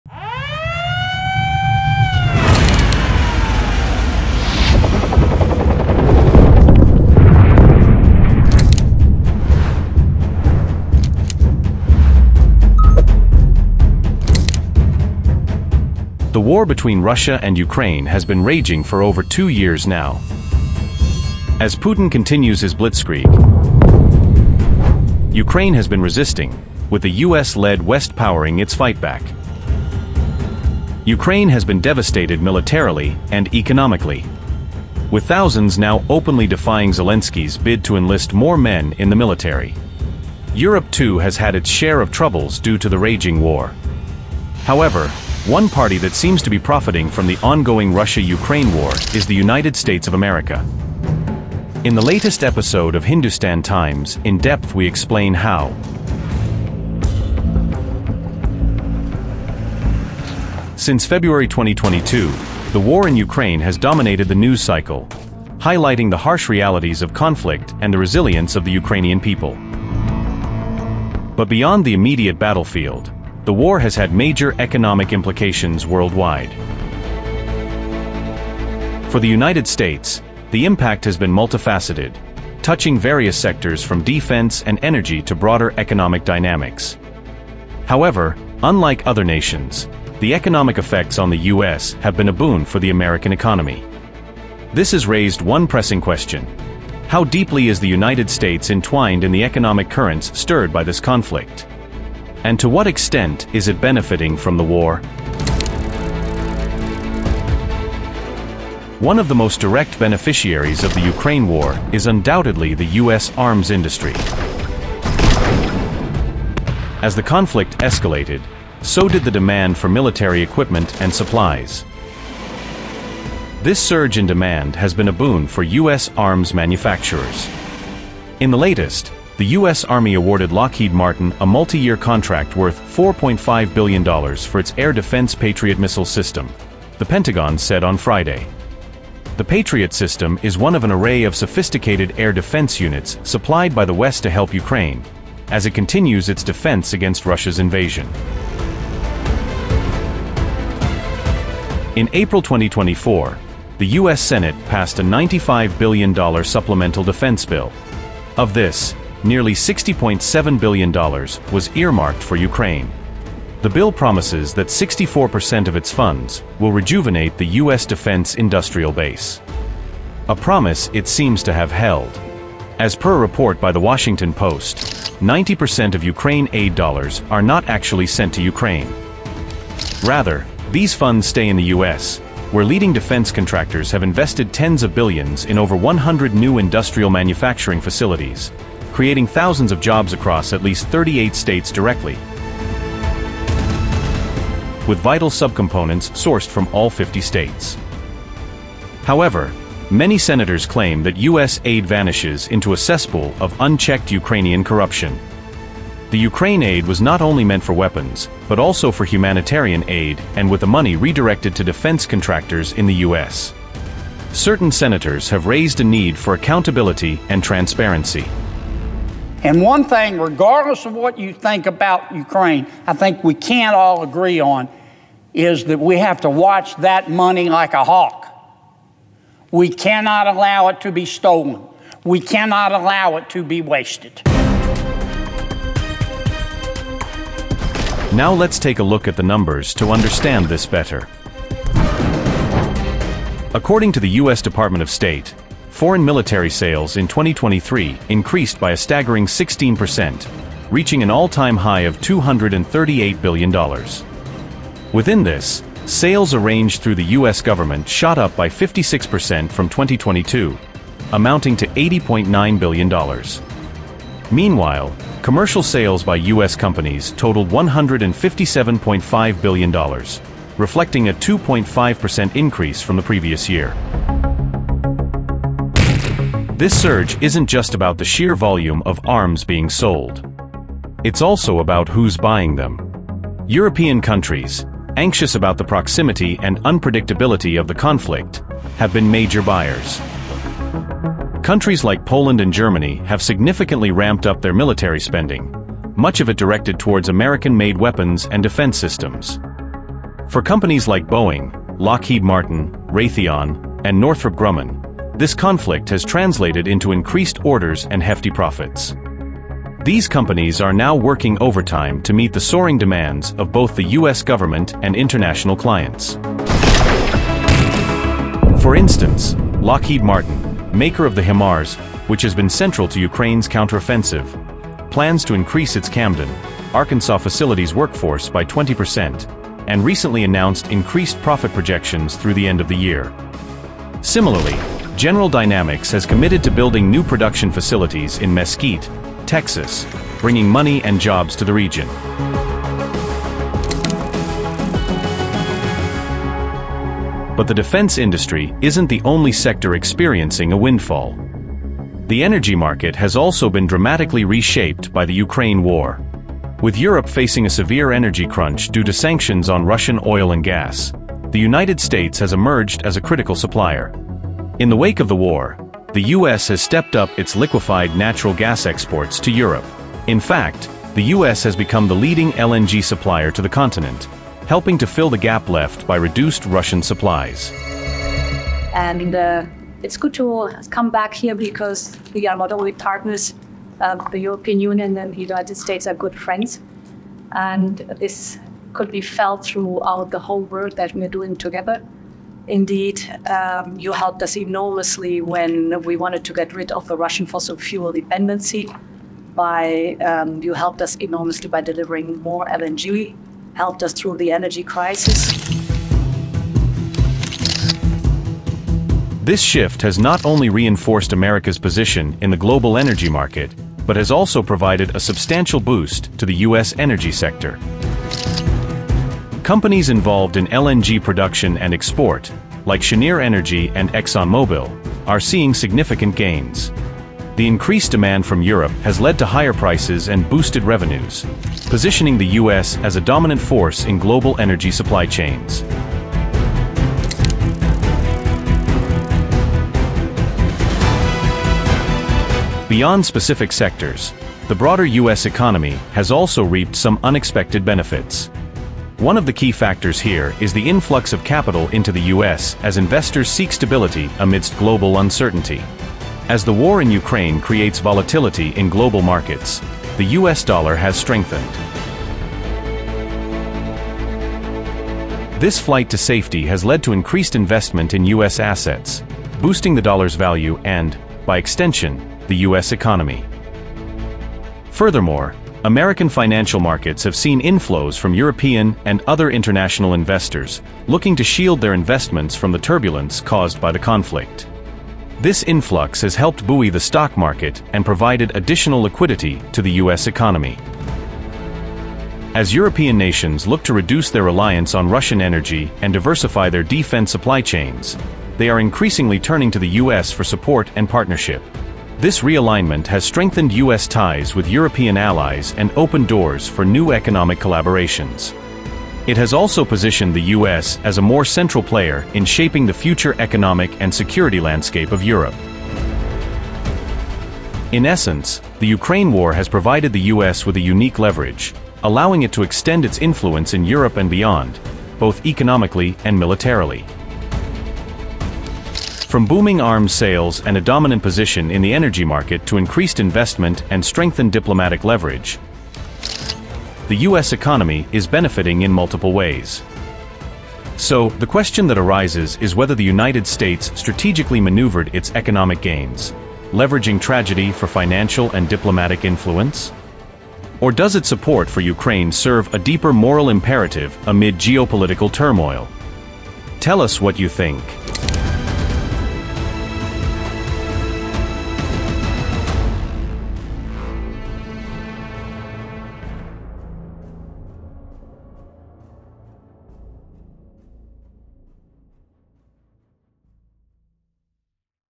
Ukraine & Europe Face Brunt Of War, But Biden’s U.S. Is Minting Money From Kyiv’s Sufferings
In the shadow of Ukraine's turmoil, the United States has seized a pivotal role, capitalizing on the conflict's economic ripples. From record-breaking arms contracts bolstering its defense industry to becoming Europe's top LNG supplier amid energy crises, America's gains are undeniable, questioning the true cost of this windfall. Is the U.S. leveraging tragedy for strategic gain, reshaping global alliances and bolstering its own economy under the guise of aid? Or does its involvement serve a deeper moral imperative, ensuring stability and security in a volatile world?

INTERNATIONAL NEWS